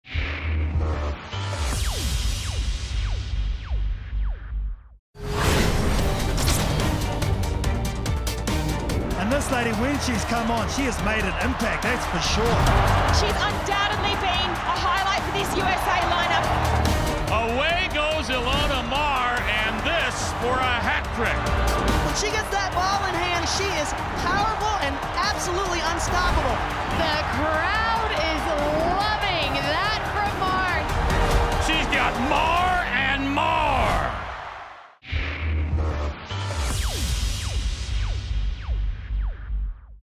One to watch: Ilona Maher
Pivotal to USA's semi-final win at the Glendale Sevens, Ilona Maher can score them from anywhere. She's your one to watch at the Dubai7s